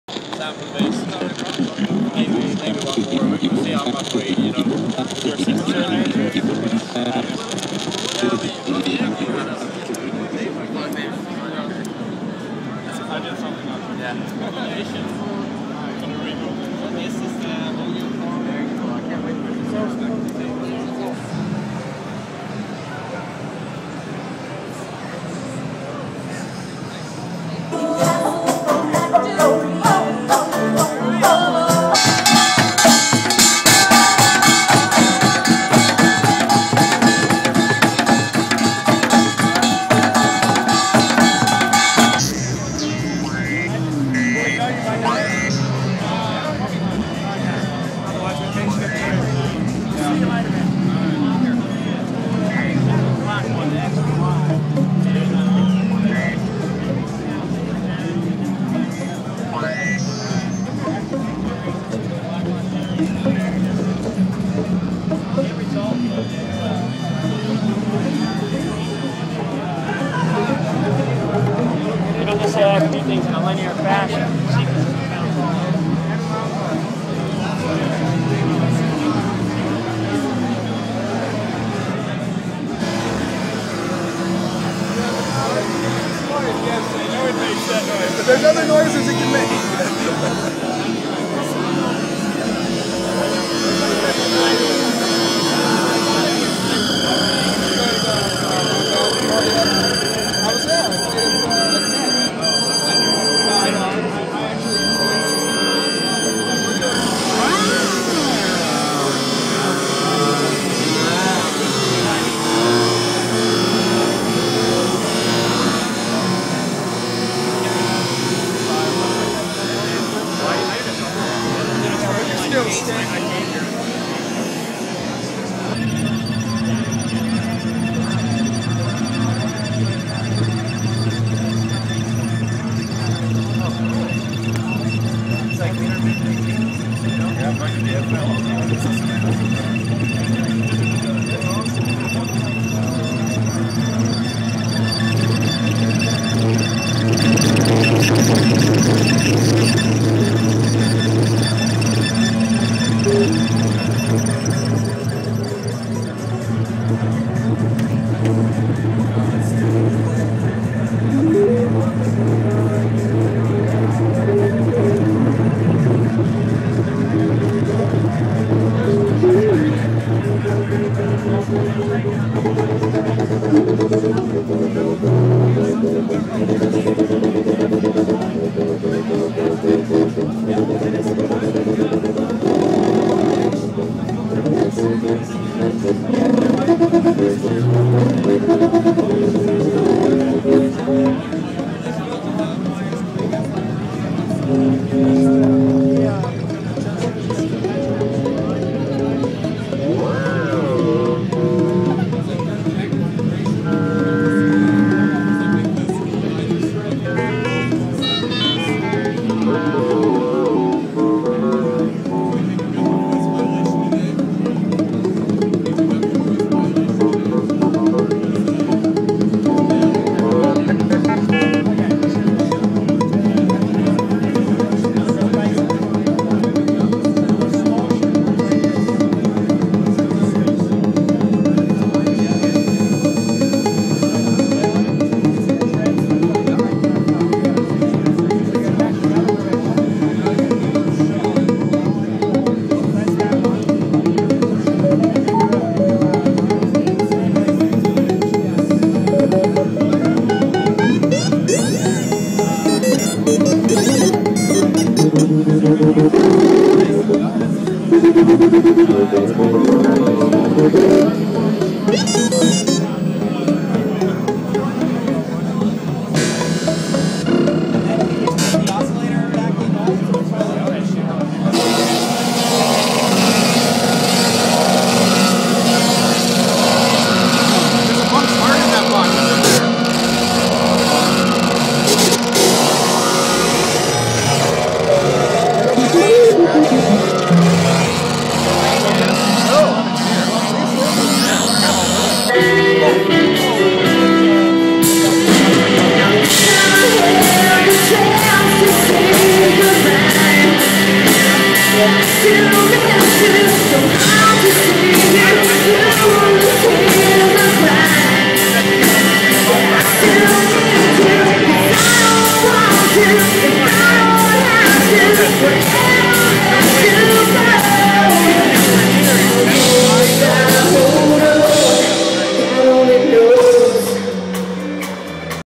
A short look @ namm 2010
Me short visit to namm show, see modular synths, anti sweat-shop guitar mfg protest (gibson fender and others), Orianthe